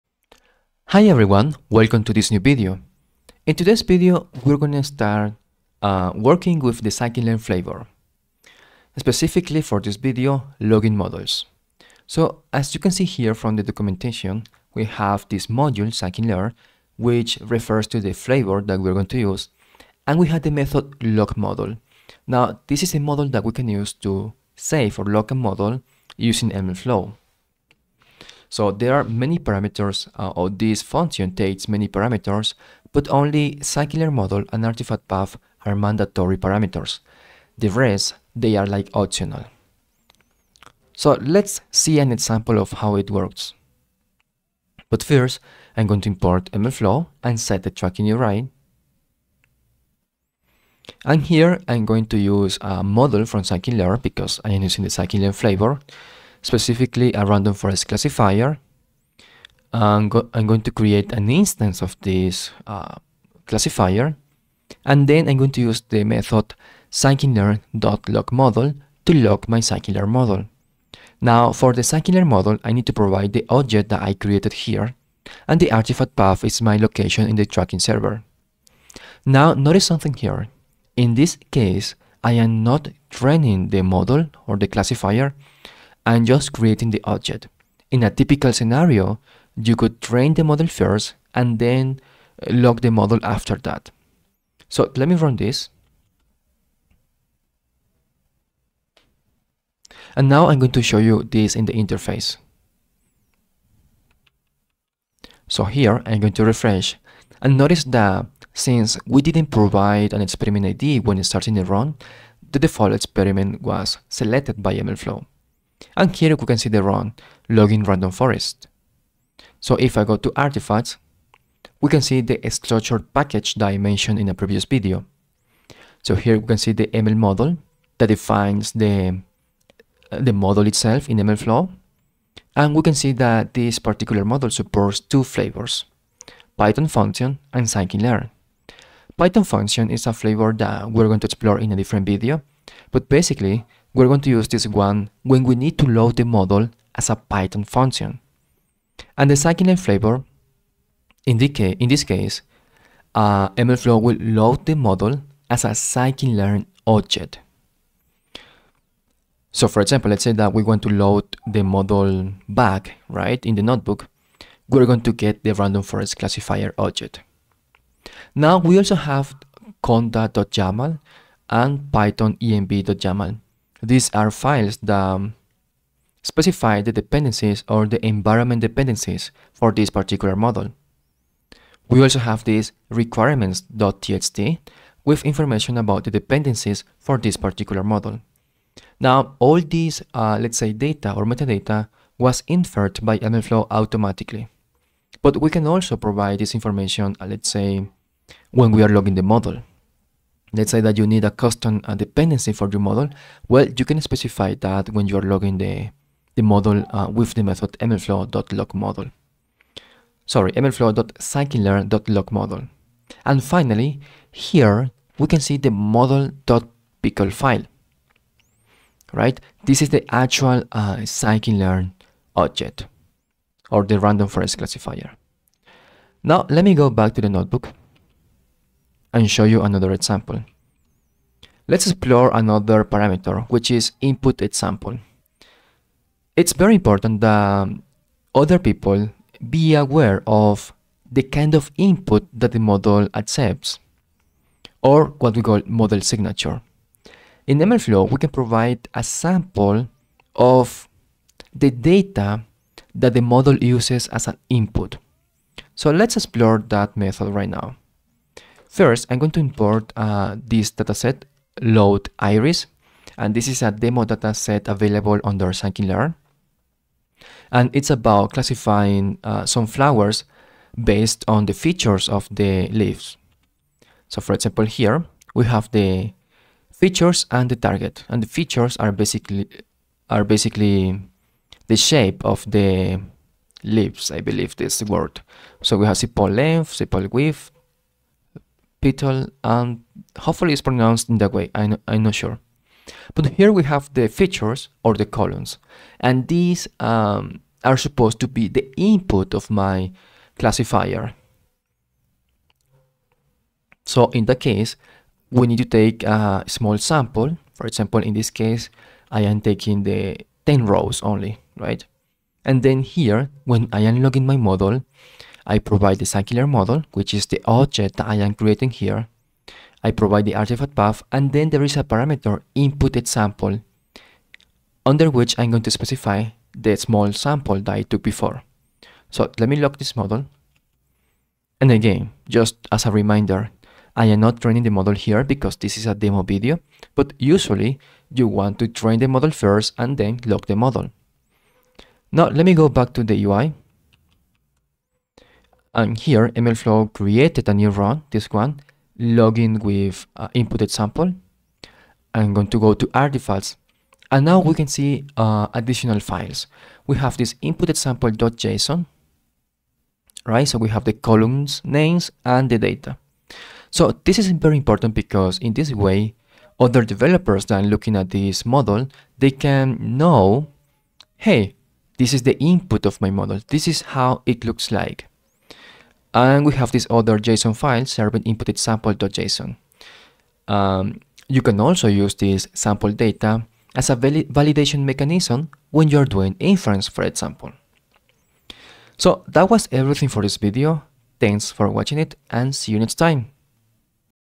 17. MLflow models. The sklearn flavor.
MLflow for Machine Learning Development is a playlist with tutorials about the MLFlow python SDK. It shows how to use the most common methods that MLflow provides to handle the Machine Learning Lifecycle.